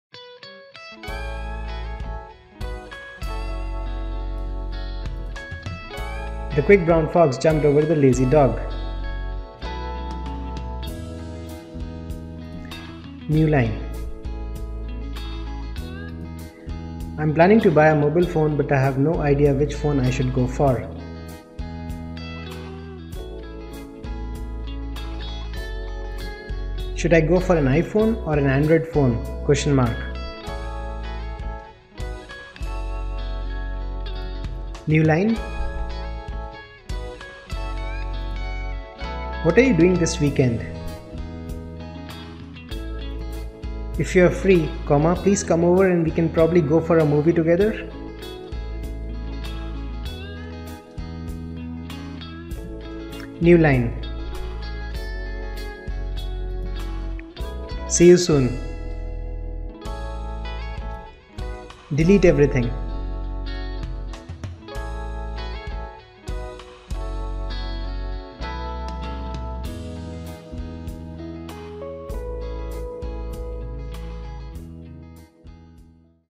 Voice Recognition using Google Chrome
Looking for free voice recognition software for your Windows or Mac computer that can accurately transcribe your spoken voice? Try the new Dictation app in your Google Chrome browser.